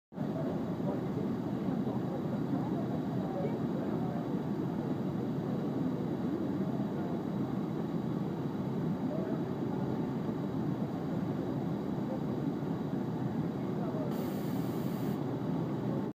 Mail sorter
This machine sorts more than 36000 pieces of mail per hour. That's 10 per second.